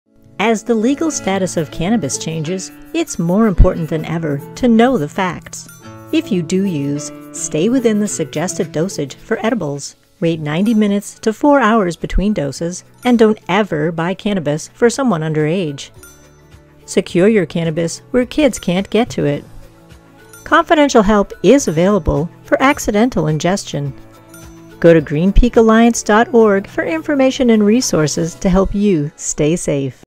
Cannabis Safety Tips
With Vermont communities making decisions about the presence of cannabis, it is more important than ever for you to know the facts so you can make informed decisions and stay safe. This quick video gives you helpful tips.